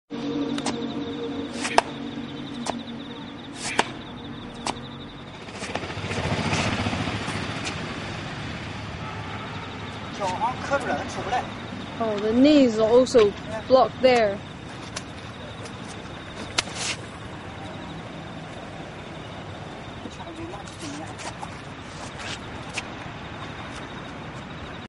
Shang Xing Yi Quan - Quick Demonstration of Basic Application
Quick Demonstration of Basic Application for Shang style Xing Yi Quan.